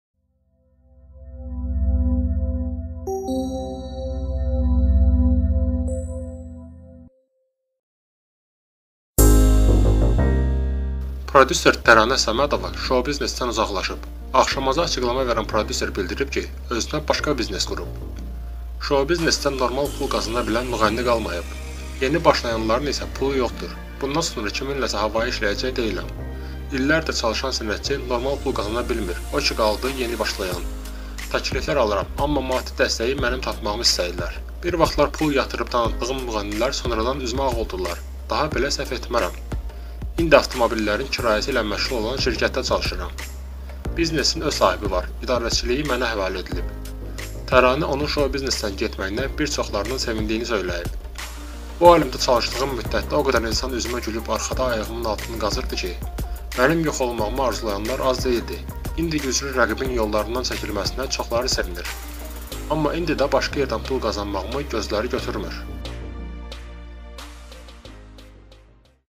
Təranə şou-biznesdən getdi, biznes qurdu: "Daha belə səhv etmərəm"
Prodüser Təranə Səmədova şou-biznesdən uzaqlaşıb.

Milli.Az xəbər verir ki, axşam.az-a açıqlama verən prodüser bildirib ki, özünə başqa biznes qurub:

"Şou-biznesdə normal pul qazana bilən müğənni qalmayıb. Yeni başlayanların isə pulu yoxdur. Bundan sonra kimləsə havayı işləyəcək deyiləm. İllərdir çalışan sənətçi normal pul qazana bilmir, o ki qala yeni başlayan. Təkliflər alıram, amma maddi dəstəyi mənim tapmağımı istəyirlər. Bir vaxtlar pul yatırıb, tanıtdığım müğənnilər sonradan üzümə ağ oldular. Daha belə səhv etmərəm.

İndi avtomobillərin kirayəsi ilə məşğul olan şirkətdə çalışıram. Biznesin öz sahibi var, idarəçiliyi mənə həvalə edib".

Təranə onun şou-biznesdən getməyinə bir çoxlarının sevindiyini söyləyib:

"Bu aləmdə çalışdığım müddətdə o qədər insan üzümə gülüb, arxada ayağımın altını qazırdı ki... Mənim yox olmağımı arzulayanlar az deyildi. İndi güclü rəqibin yollarından çəkilməsinə çoxları sevinir. Amma indi də başqa yerdən pul qazanmağımı gözləri götürmür".